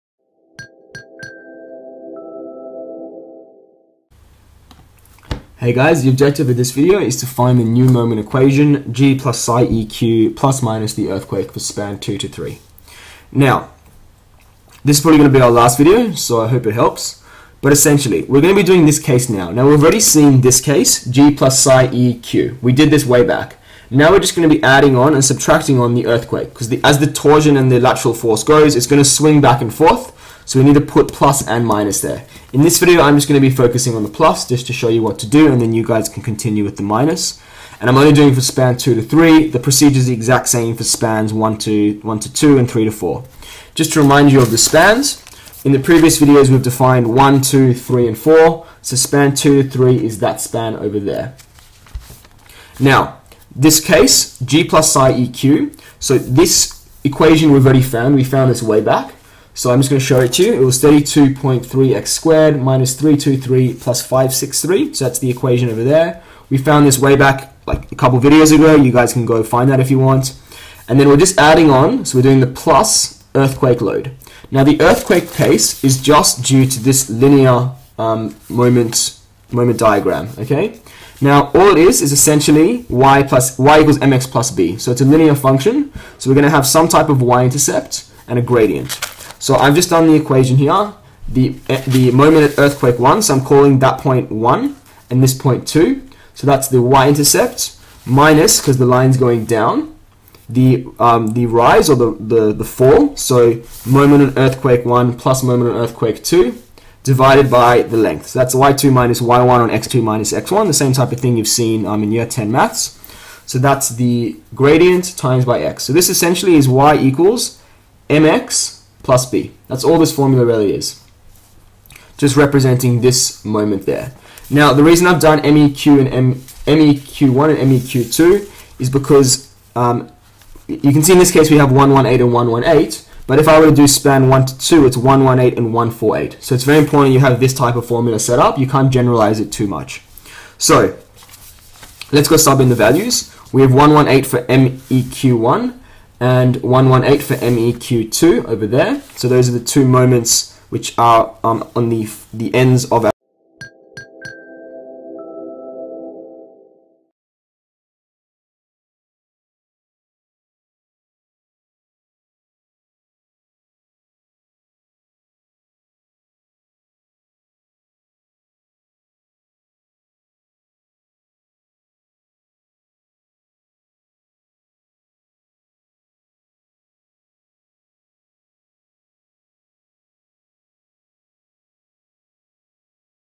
Moment Equation and Curve | Reinforced Concrete Design
for more FREE video tutorials covering Concrete Structural Design

The objective of this video is to develop a new moment equation for the span 2-3 for a given load combination; consequently draw the bending moment curve for the developed equation of moment. First part of the video explains the difference found in this load combination and its nature which is nothing but the effects of an earthquake loading. The difference is that the earthquake loading will be mutually added and subtracted alternatively with the load combination of G + EQ as the loading due to earthquake results swing back and forth intermittently.

Next, the video uses the schematic diagram of moments that has been found at the previous video 8.23 to develop the new moment equation for the considered load combination; subsequently gives overview how to find the values of integral constants using different cases to get the final equation. Later, the video determines y values for different x values & plots them to get the bending moment curve for the considered load combination over the earlier developed bending moment diagram.